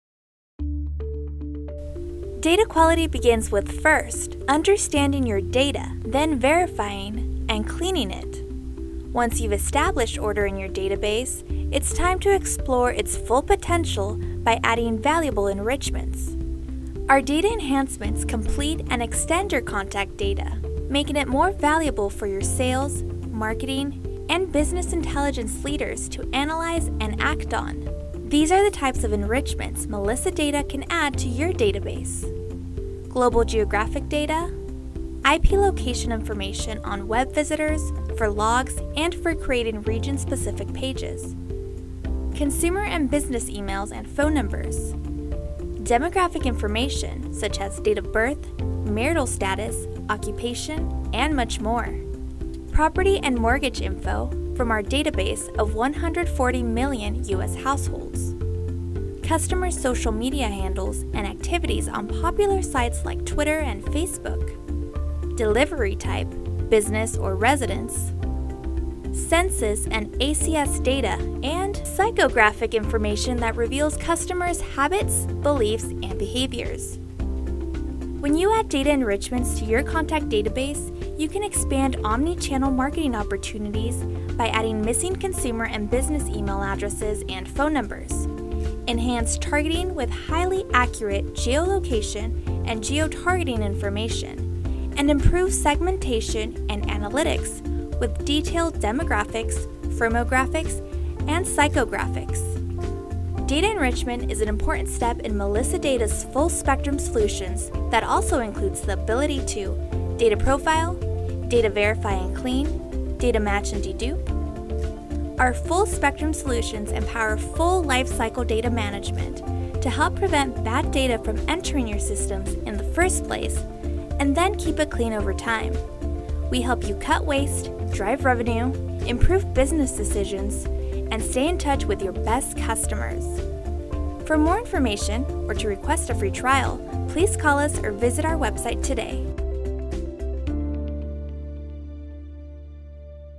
Data Enrichment
Data enrichment helps complete your customer data, providing valuable information for sales and marketing teams and Business Intelligence leaders to analyze and take action. Begin data enrichment once you have made sure contacts are verified and clean. Melissa Data enrichments include global geographic data; IP location info on Web visitors; consumer and business emails and phone numbers; demographic data (date of birth, occupation, marital status and more); customer social media handles from  20 social media sites (Twitter and Facebook, etc); and psychographic information on customer habits, beliefs, and behavior. Data enrichment will help you expand omnichannel marketing opportunities and improve segmentation.